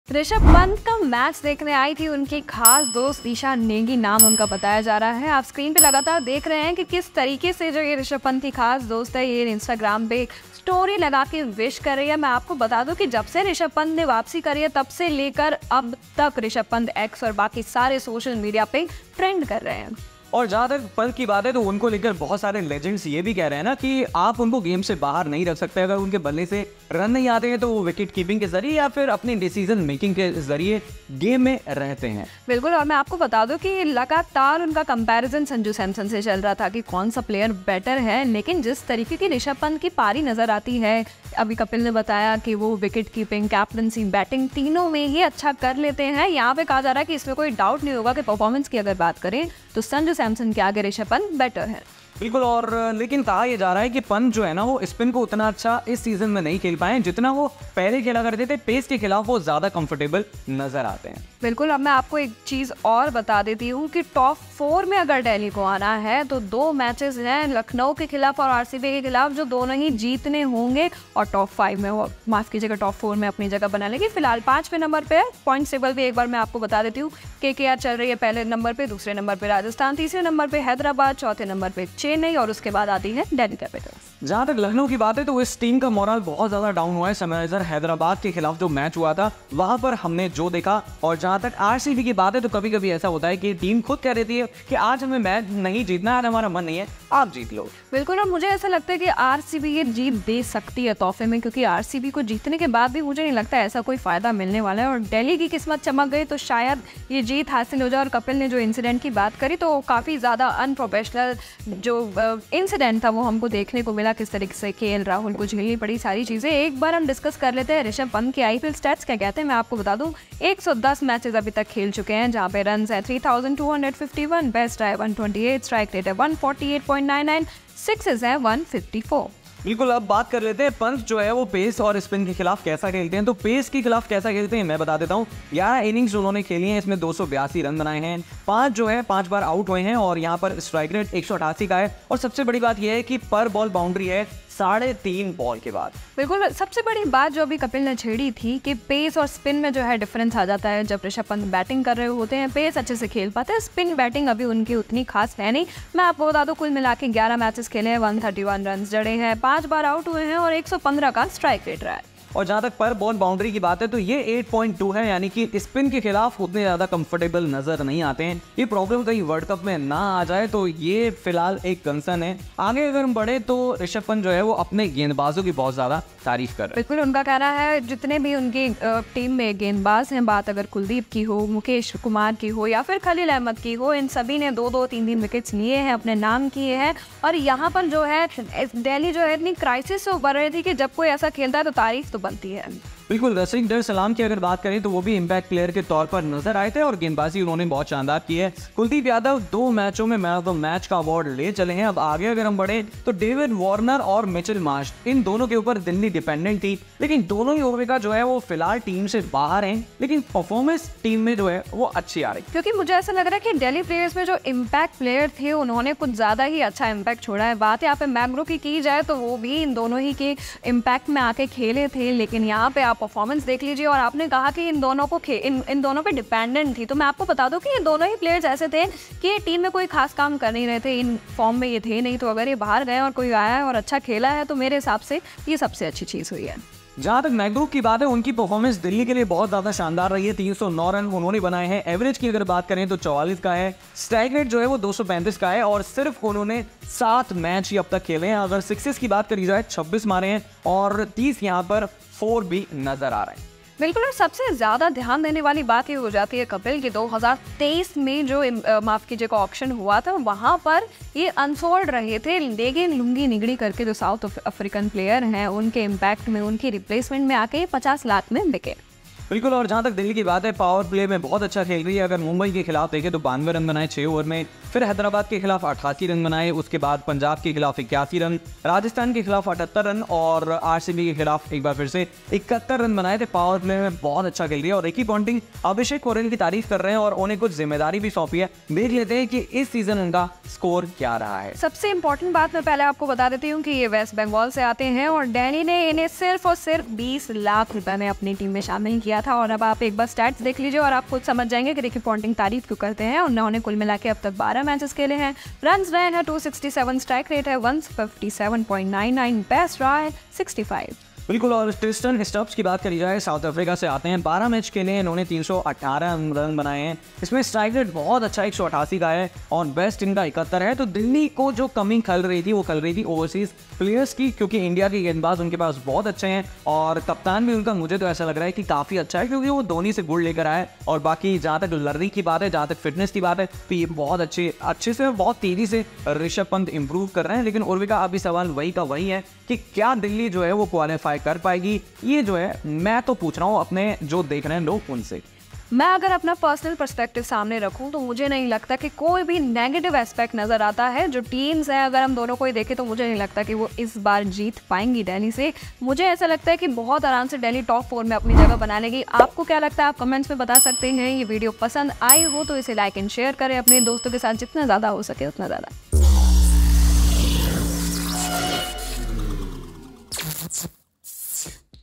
Rishabh Pant's Girlfriend's Victory Celebration: Inside Story!
Dive into the latest cricket buzz with a comprehensive roundup of IPL 2024 highlights and insights! Rishabh Pant's girlfriend, Isha Negi, recently shared her jubilation over Delhi Capitals' victory against RR on Instagram, adding to the celebratory atmosphere surrounding the team. Pant, the skipper of Delhi Capitals, continues to be a focal point of discussion, not only for his explosive batting performances but also for his exceptional skills behind the stumps. While Delhi Capitals' success persists despite the absence of key players like David Warner and injuries to others like Mitchell Marsh, lesser-known players like McGurk are stepping up to the plate and making significant contributions. In the power play, Delhi Capitals have shown prowess, with impressive scores against formidable opponents like MI, SRH, and PK.The debate surrounding Pant's overall performance compared to other stalwarts like Sanju rages on, fueled by statistical insights into Pant's gameplay against both pace and spin bowling. Additionally, Abhishek Porel, hailing from West Bengal, has emerged as a valuable asset for Delhi Capitals, delivering consistent performances on the field. Porel's inclusion in the team for a mere 20 lacs has proven to be a bargain for Delhi Capitals, showcasing the team's shrewd decision-making during the IPL 2024 auction.With the IPL standings reflecting a close competition among teams like KKR, RR, SRH, and CSK, every match becomes crucial in determining the playoff contenders.